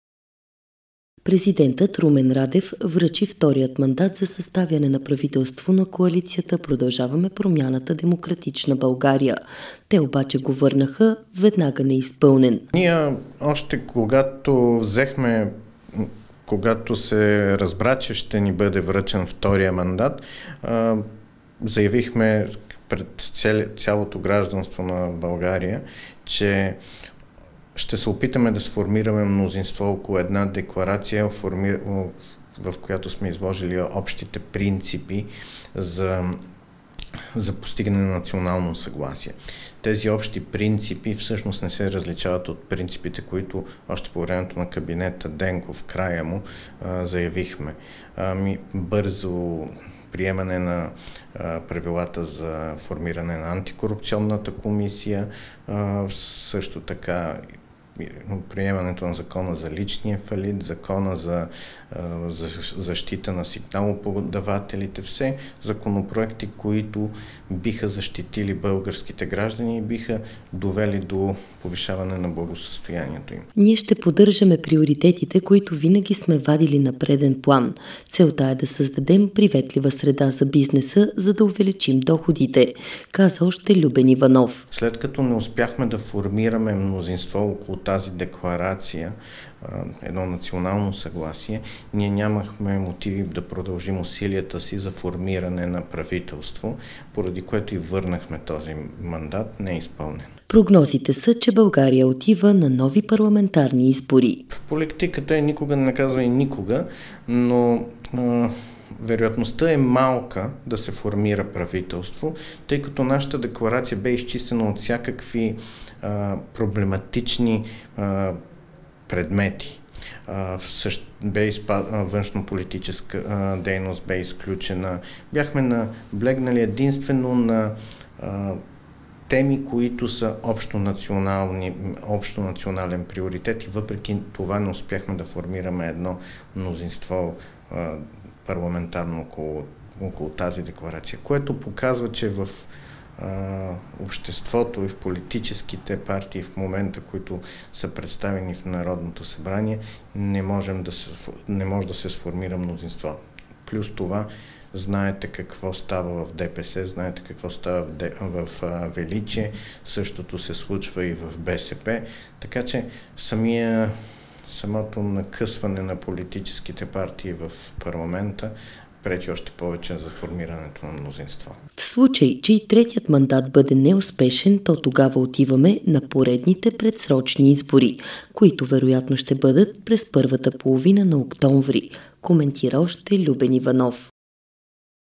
ПП – ДБ върна на президента папката за втори мандат
Президентът Румен Радев връчи втория мандат за съставяне на правителство на Коалицията „Продължаваме промяната – Демократична България”. Те обаче го върнаха веднага неизпълнен.